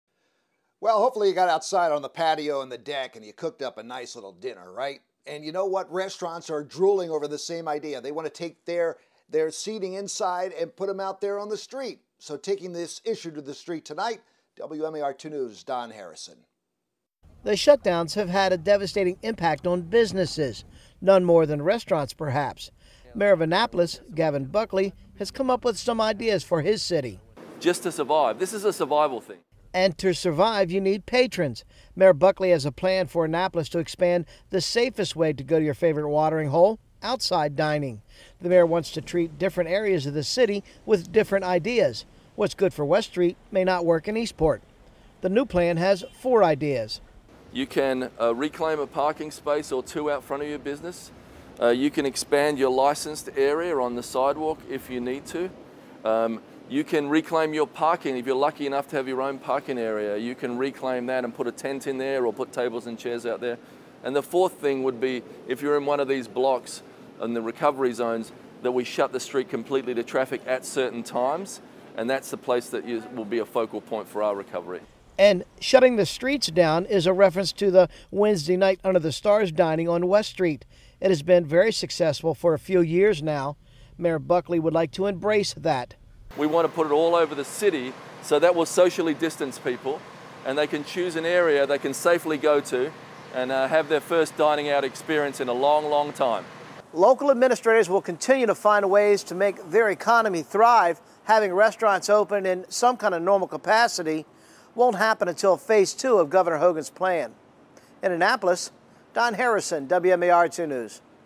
Mayor of Annapolis proposes safe ways to reopen restaurants
Social restrictions are lifting and people are itching to get to a restaurant. Restaurant owners are anxious for customers to come back as well.